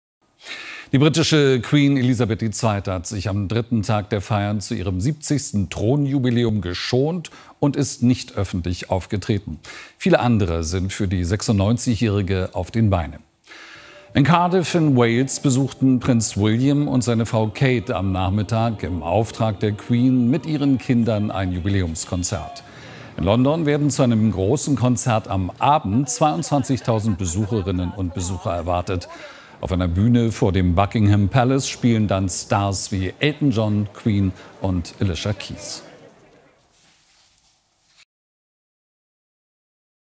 Queens Special !
siehe Infokarte

70. Thronjubiläum der Queen: Konzerte in Wales und London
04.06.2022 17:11